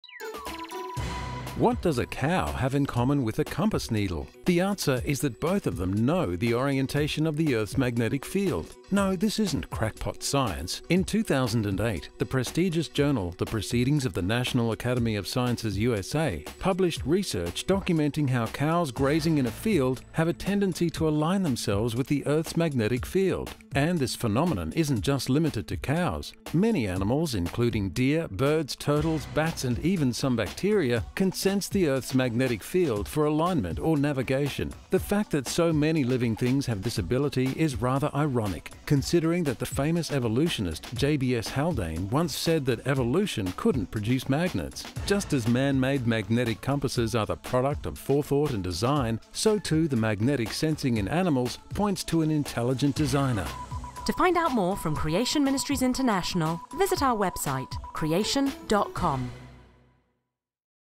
Cows acting like compasses
What does a cow have in common with a compass needle?  The answer is that both of them ‘know’ the orientation of the earth’s magnetic field.
No, this isn’t crackpot science. In 2008, the prestigious journal, the Proceedings of the National Academy of Sciences USA, published research documenting how cows grazing in a field have a tendency to align themselves with the earth’s magnetic field.
And this phenomenon isn’t just limited to cows; many animals including deer, birds, turtles, bats, and even some bacteria, can sense the earth’s magnetic field for alignment or navigation.
The fact that so many living things have this ability is rather ironic, considering that the famous evolutionist J.B.S Haldane once said that evolution couldn’t produce magnets.
Just as man-made magnetic compasses are the product of forethought and design, so, too, the magnetic sensing in animals points to an intelligent designer.